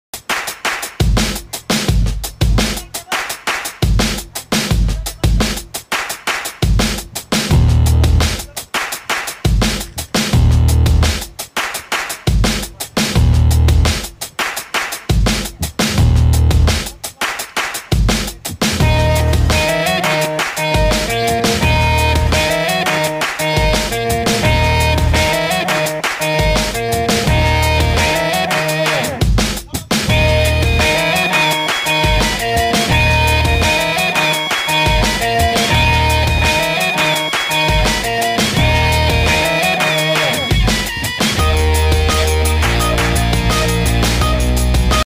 BMW 320d Towing down from truck
How to tow four wheeler from towing van.